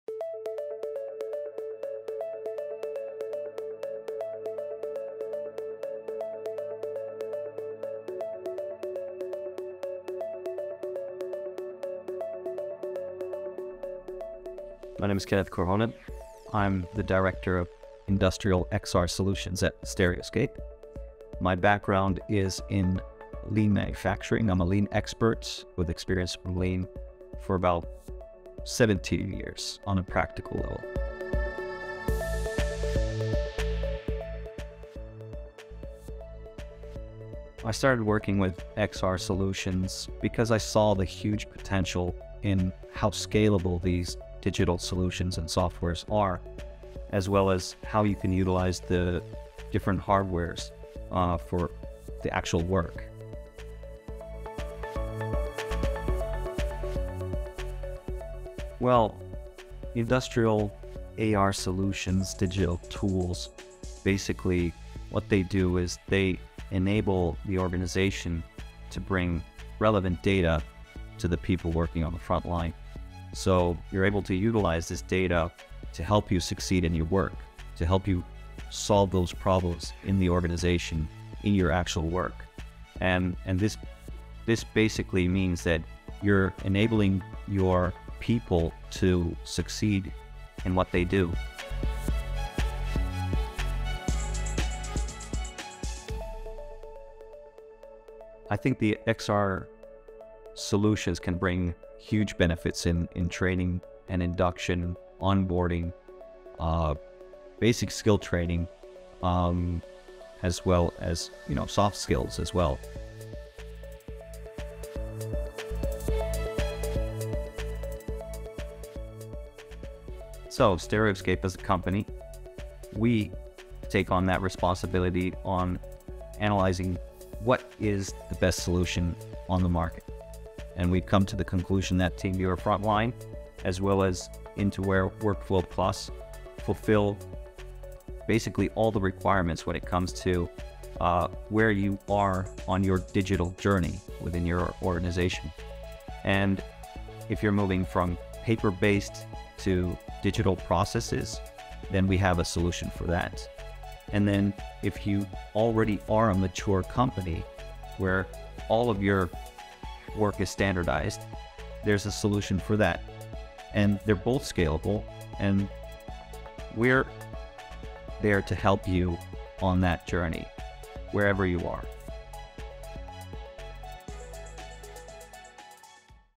[Interview] Stereoscape’s Kenneth Korhonen and the benefits of AR tools for industrial companies
Interactive and augmented reality solutions like TeamViewer’s Frontline and Intoware’s WorkfloPlus are transforming frontline work. Stereoscape’s Industrial XR Solutions Director Kenneth Korhonen talks about AR tools and argues that they are a natural extension of lean culture, helping create more autonomous and committed workers and ultimately creating value for organisations and their clients.